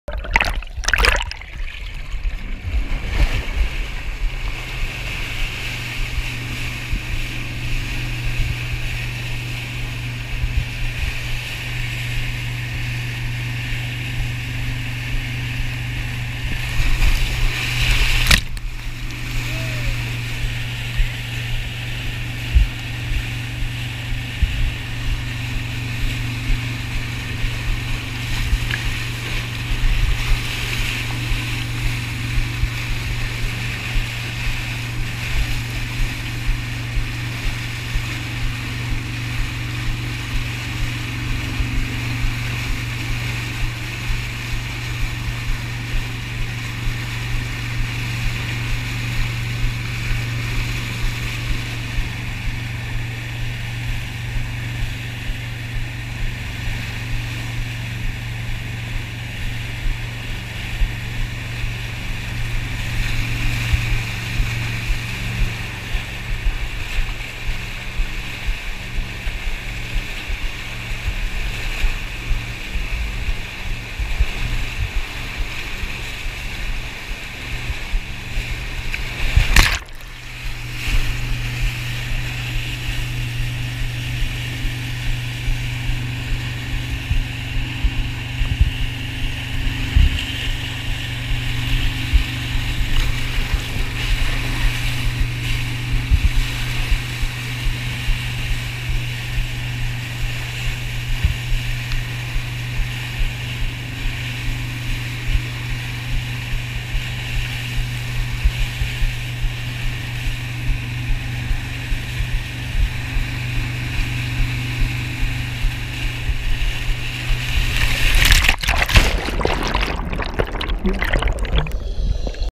Wake Surfing On Smith Lake
Wes showing me how to wake surf on Smith Lake July 5th 2015